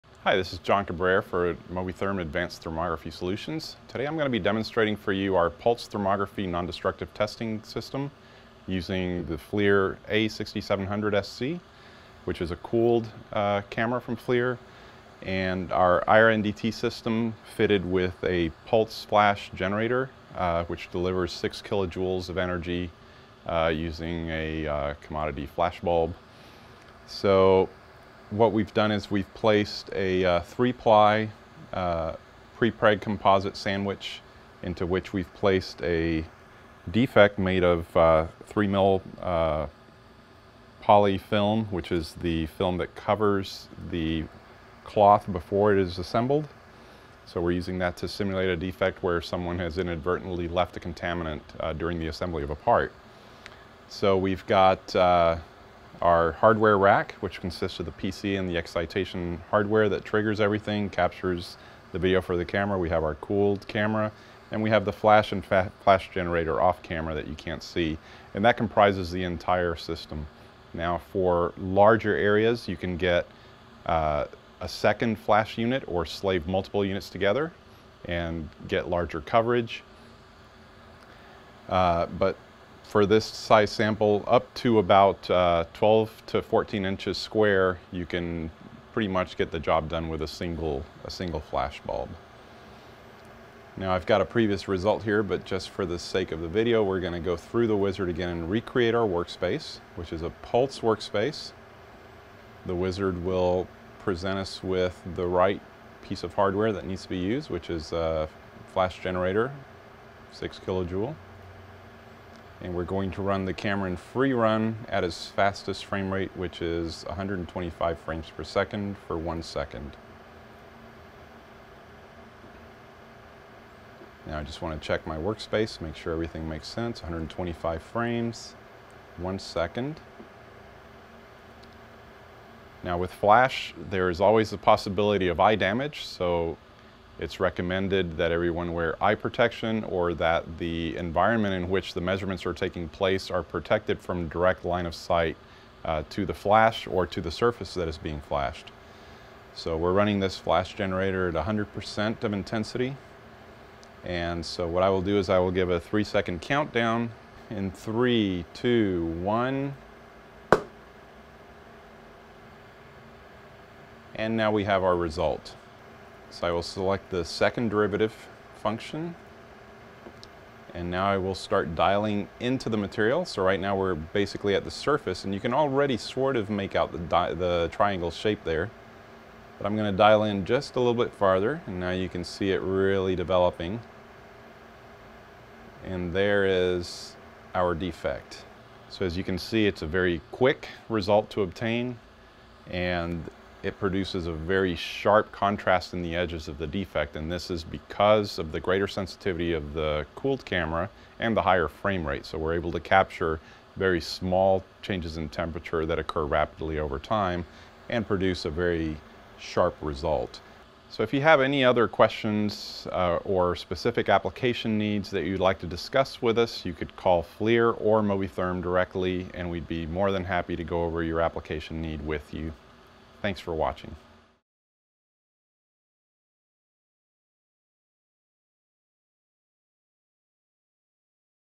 Flash Thermography for Non-Destructive Testing
MoviTHERM's Flash Thermography solutions allow for non-destructive evaluation of materials. Flash Thermography is often used for defect detection in metals and carbon composites for aerospace materials.

A Xenon flash lamp discharges several kilo Joules of energy in about 2 milliseconds and causes the surface of the material to heat up by a few degrees. A thermal camera takes a sequence of images monitoring the cool down behavior of the material.

Non-Destructive Test Software then performs a complex analysis of the image sequence and turns differences in the rate of cooling into contrast changes (darker and lighter areas) in the result image or image sequence. An on-screen slider lets the user "dial" through different depths to inspect for defects.

Flash Thermography allows for rapid non-destructive evaluation of carbon composite structures.